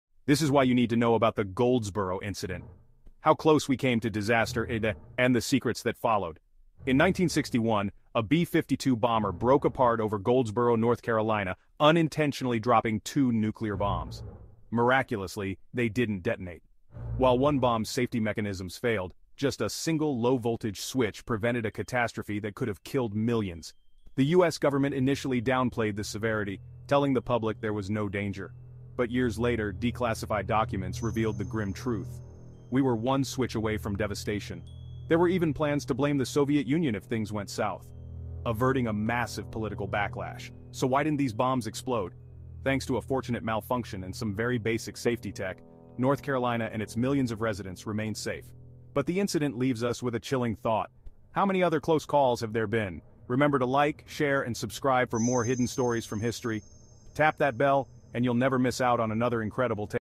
Unexploded Nuclear Disaster: the Goldsboro Close Call in North Carolina
Uncover the chilling truth behind the Goldsboro incident of 1961, where a B-52 bomber broke apart, unintentionally dropping two nuclear bombs over North Carolina. This video reveals declassified government documents that expose the severity of the near catastrophe that was downplayed by the U.S. government. Find out how a fortunate malfunction and basic safety technology saved the day, preventing the bombs from exploding. As we delve into this incredible event, we'll also ponder over other close calls throughout history.

The Goldsboro Close Call: Unexploded Nuclear Disaster